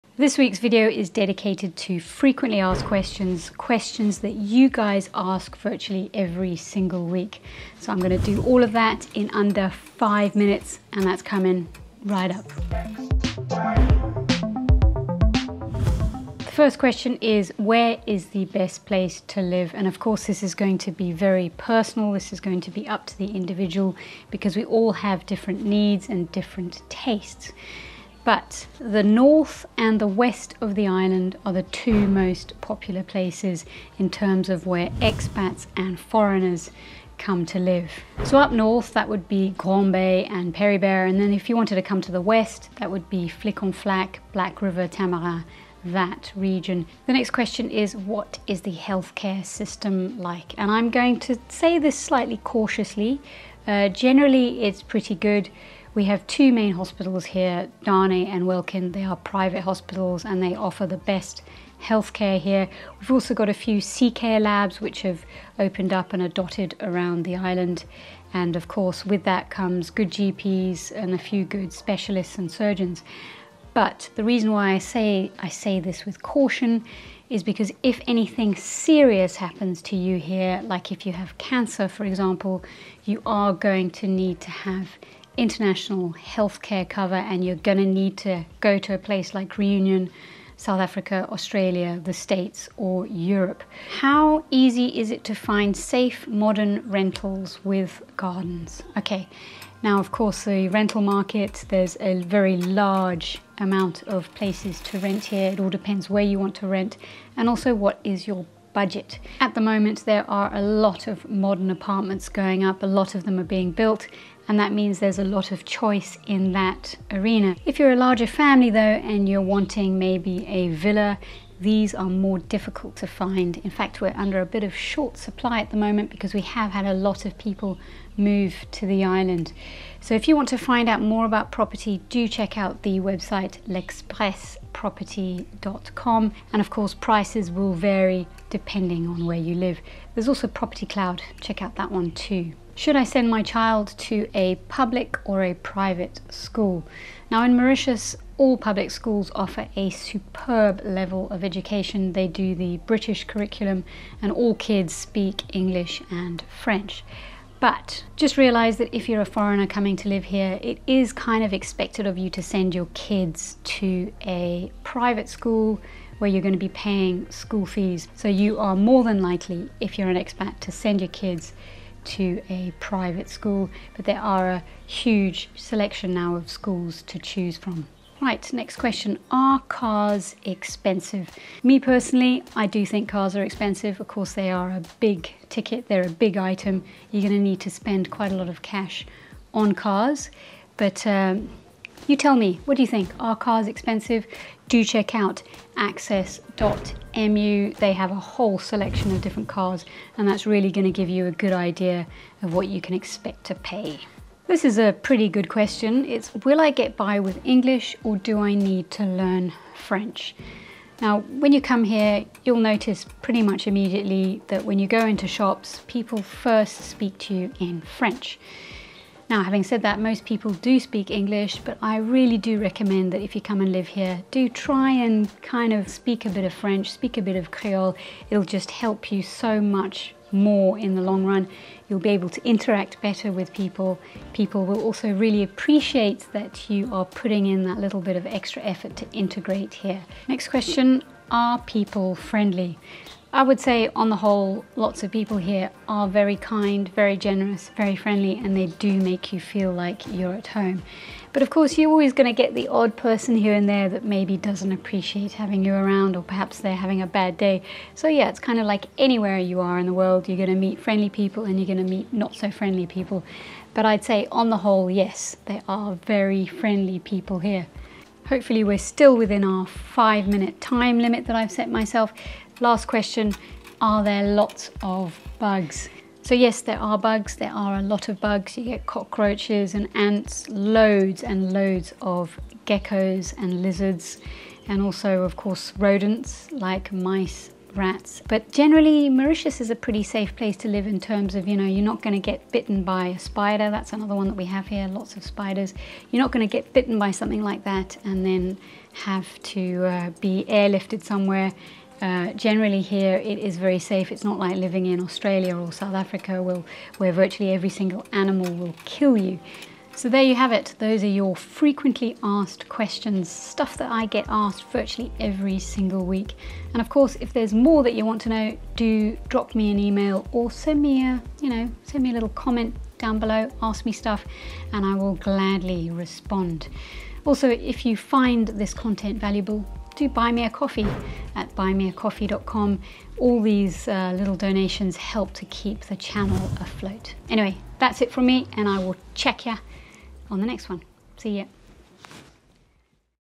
Your FREQUENTLY ASKED QUESTIONS about MAURITIUS
In this video I tackle some of your frequently asked questions about Mauritius to help you get a better understanding of certain aspects of this island.

Please note that this advice that I give is all based upon my own expereince of living here.